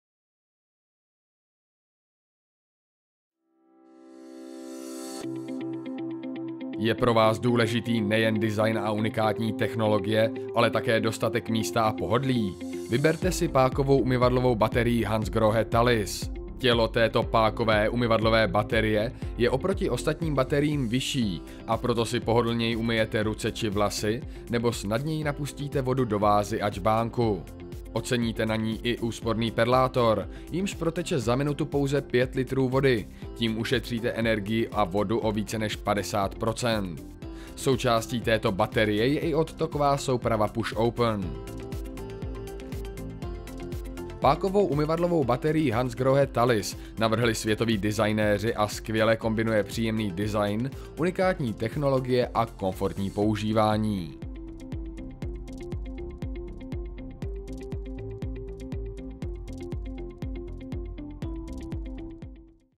hansgrohe | Talis E | Páková umyvadlová baterie | 71711000
Hledáte úspornou vodovodní baterii s nadčasovým designem? Vyberte si pákovou umyvadlovou baterii Hansgrohe Talis E.

S technologií Hansgrohe LowFlow jí za minutu proteče pouze 3,5 litrů vody, čímž ušetříte téměř 80 % vody a energie oproti běžným bateriím.

Nezaměnitelný charakter získává páková umyvadlová baterie Hansgrohe Talis E díky svému nadčasově elegantnímu vzhledu, v němž se snoubí organické tvary, přesné hrany a hladké plochy.